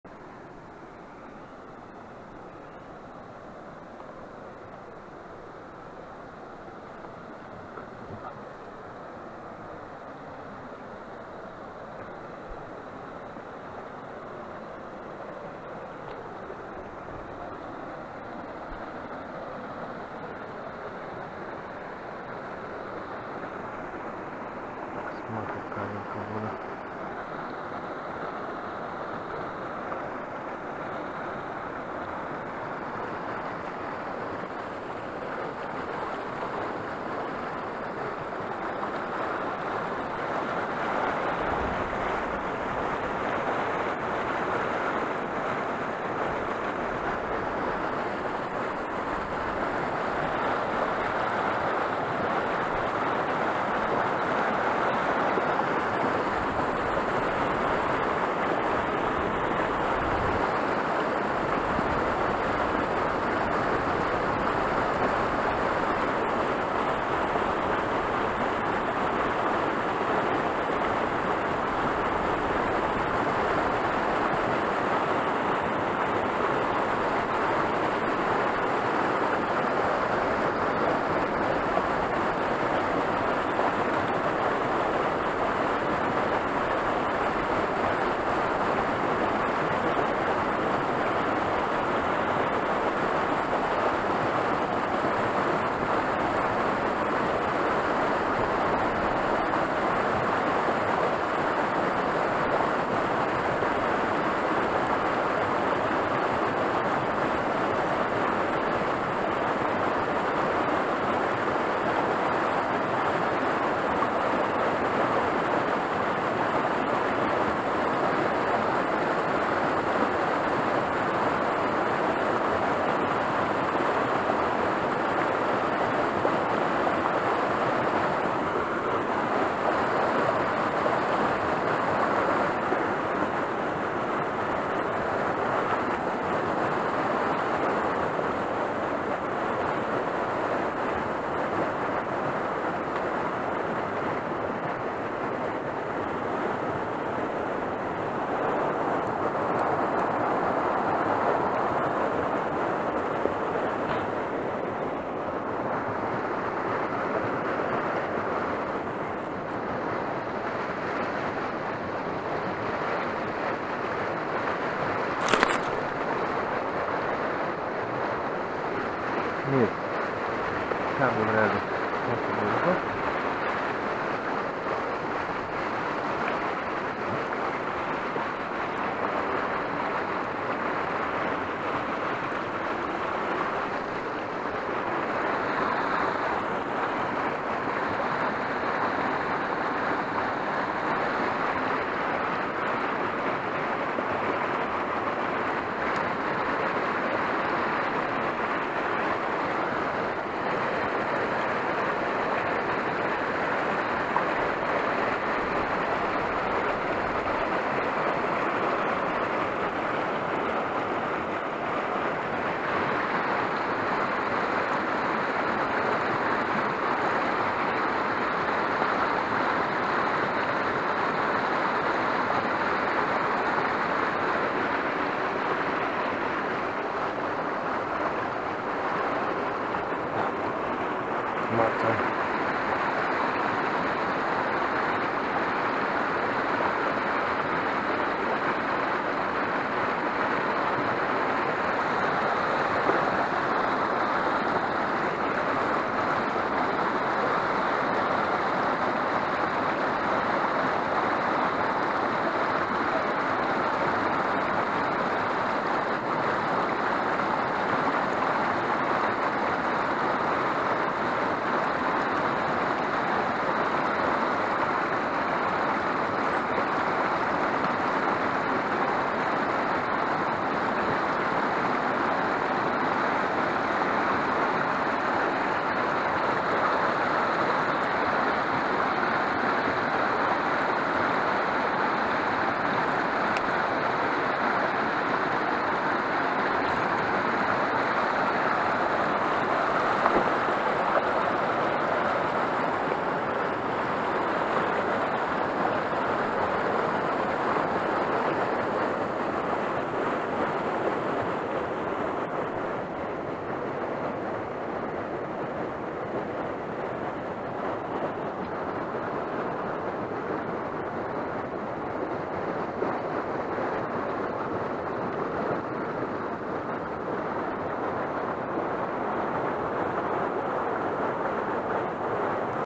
This is life:Asp vs Molix Shaman Popper#77
Rod: Harrison VHF 2.70m 30-75gr.
Reel: Shimano Twin Power SW5000XBG
Braid Line:Daiwa Tournament 8 Braid EVO 0.20mm
Flurocarbon: Savagear Soft 0.46mm
Lure:Molix Shaman Popper